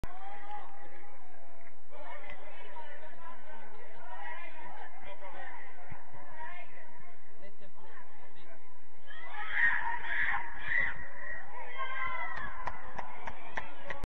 Missing a goal
From 2022 season, Raplamaa JK Reserve team a missing scoring.